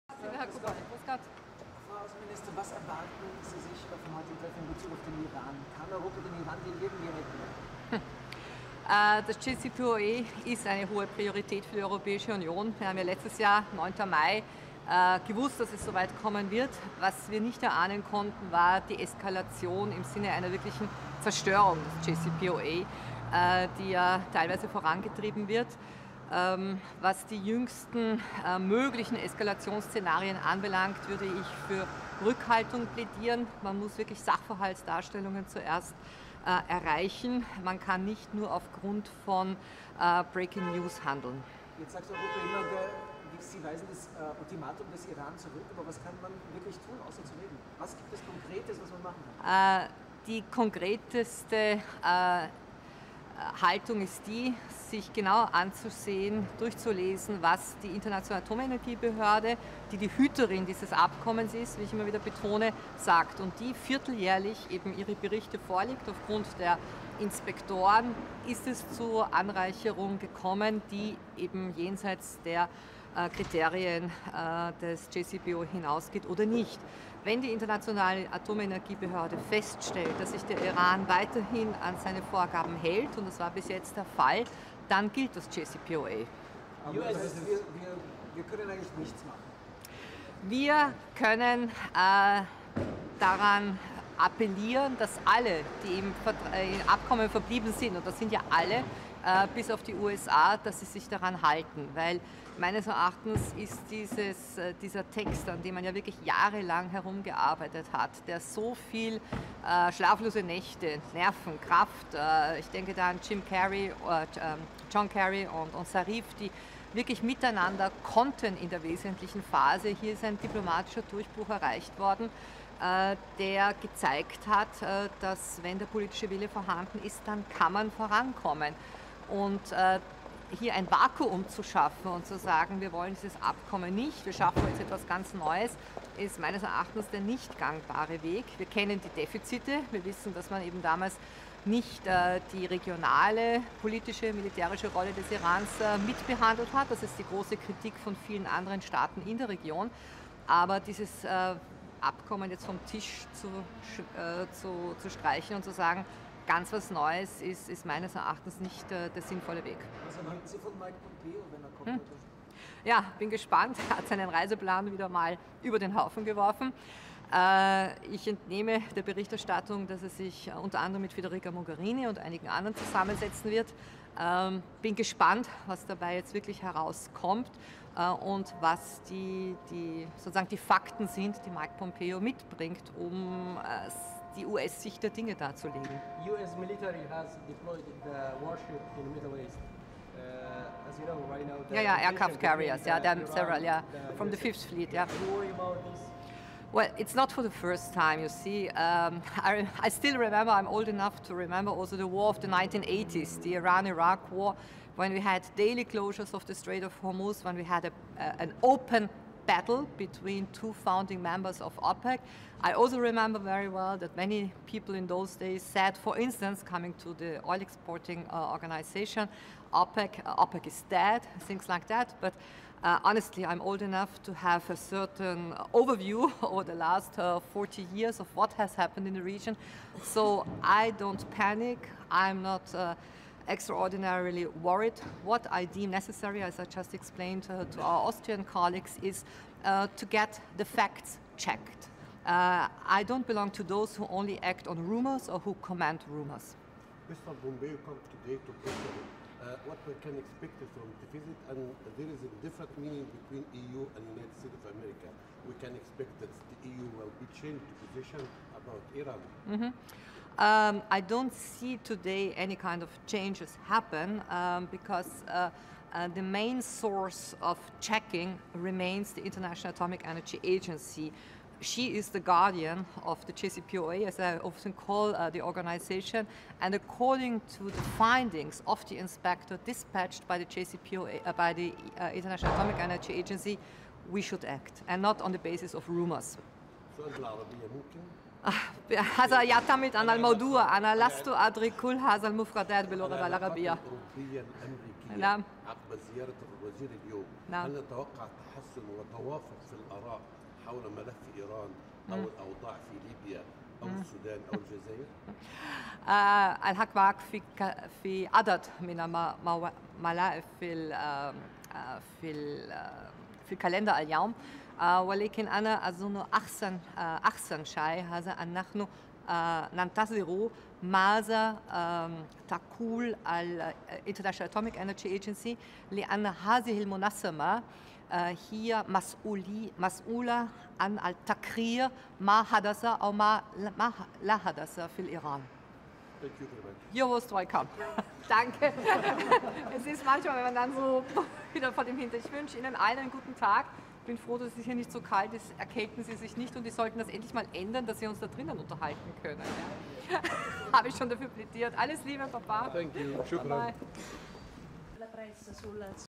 13.05.2019 - Statement Karin Kneissl - GAERC - Iran/JCPOA/USA/Pompeo/Flugzeugträger/Naher Osten/OPEC
Pressestatement der österreichischen Außenministerin vor dem Rat der Außenminister der Europäischen Union in Brüssel vom 13. Mai 2019, 10.25 Uhr